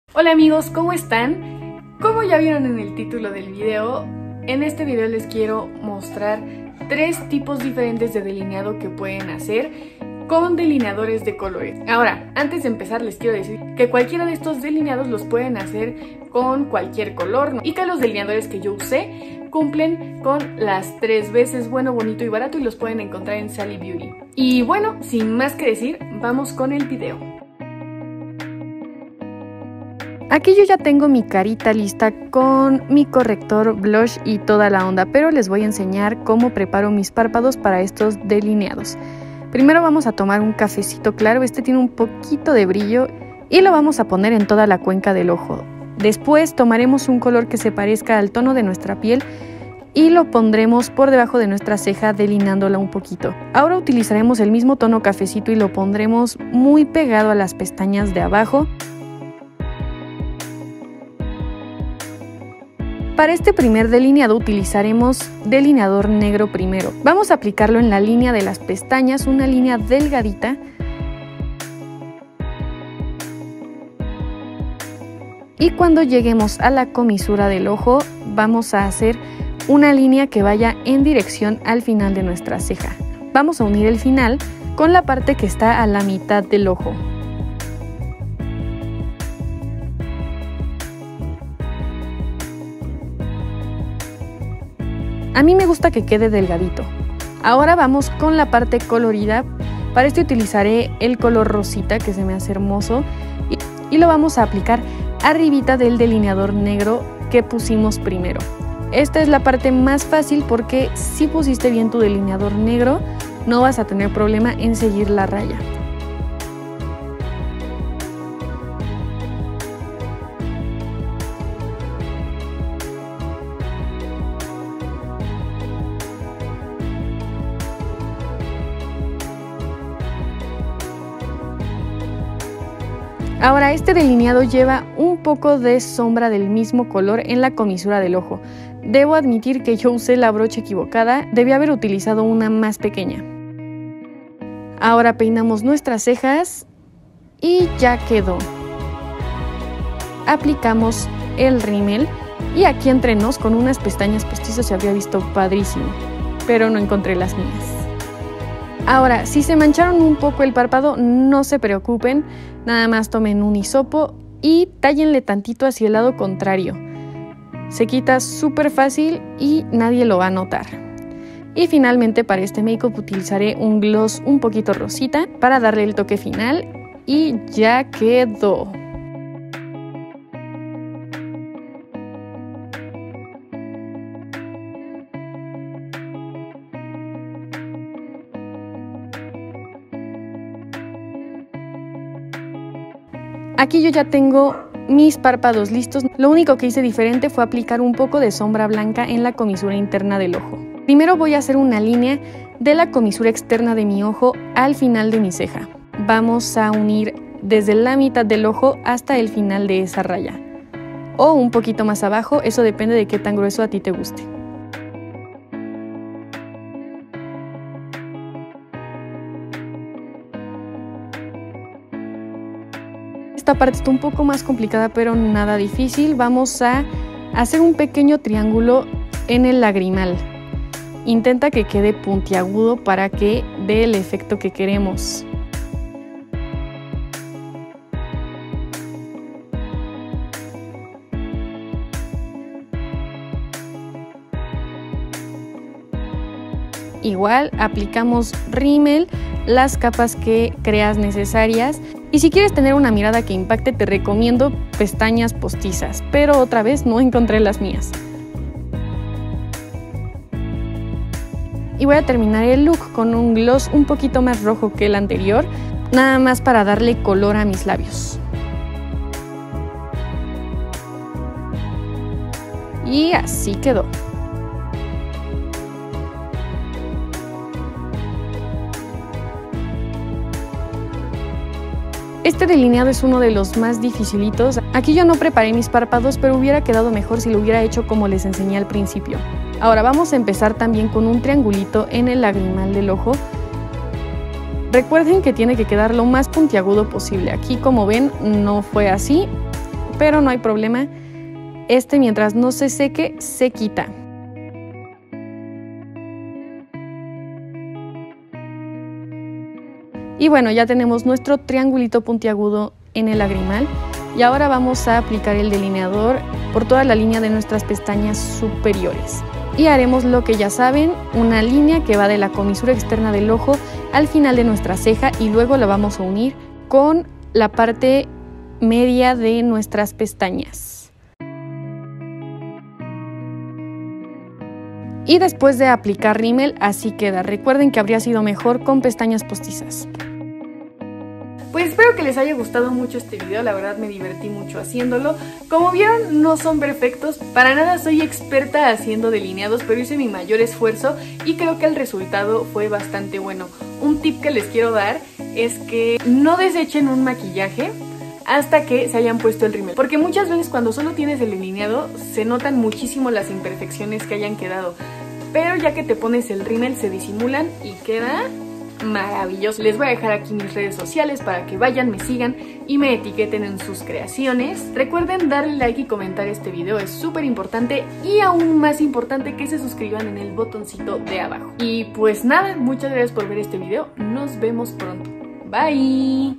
DELINEADORES DE COLORES
En este video les enseño 3 diferentes formas de maquillarte con delineadores de colores.
No olviden seguirme en insta: @marianabotello21